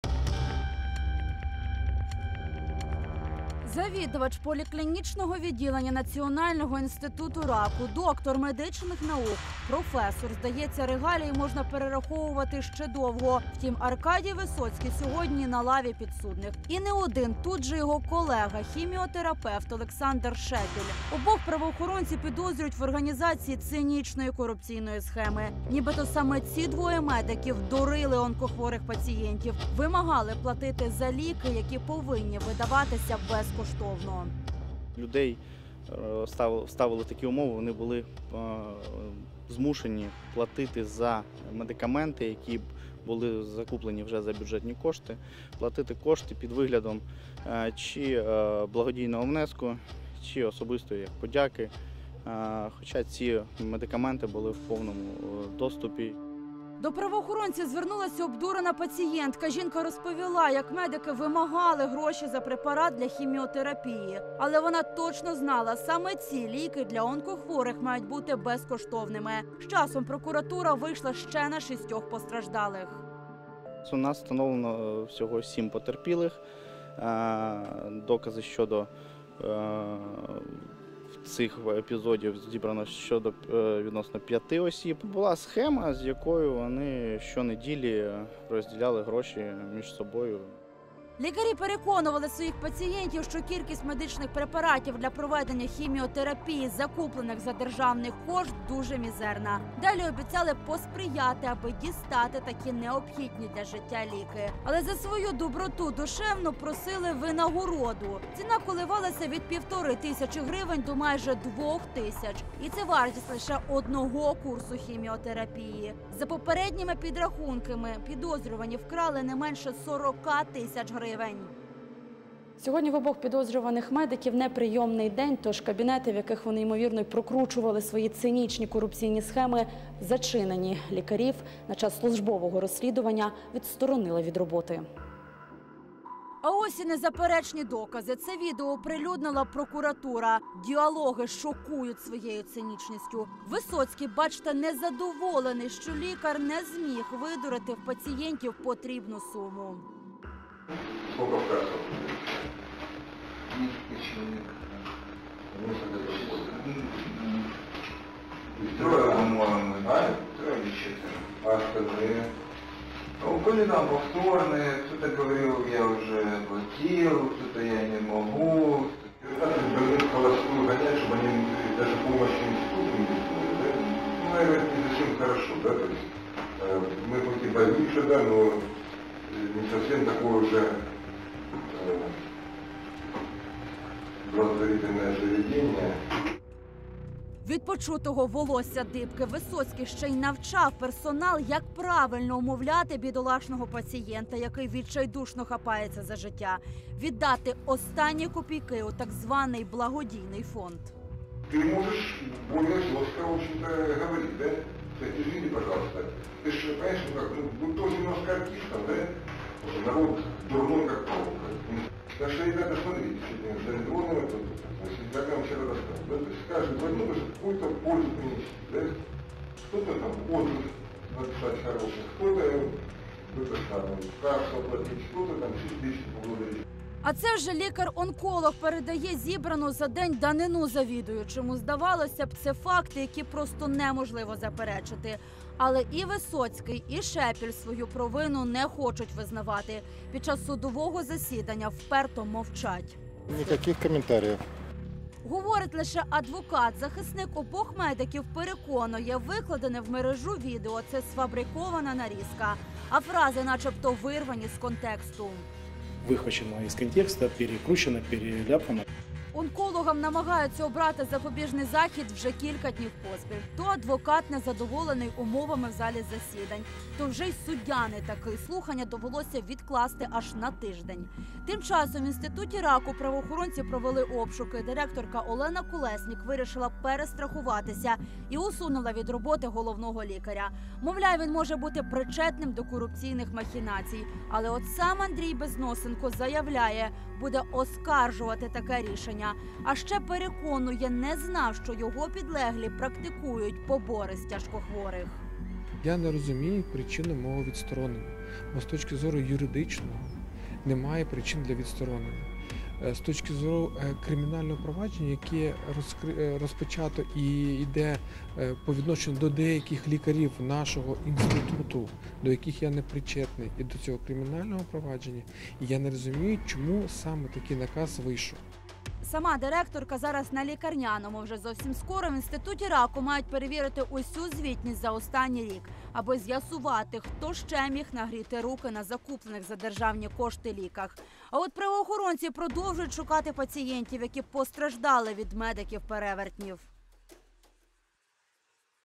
Медики-перевертні: лікарів столичної медустанови звинувачують у хабарництві
Доктор медичних наук на лаві підсудних. Він не один. Поряд -колега. Їх підозрюють в організації цинічної схеми, за якою пацієнти вимушені були платити гроші за безкоштовні ліки. Правоохоронці продовжують шукати постраждалих від дій медиків.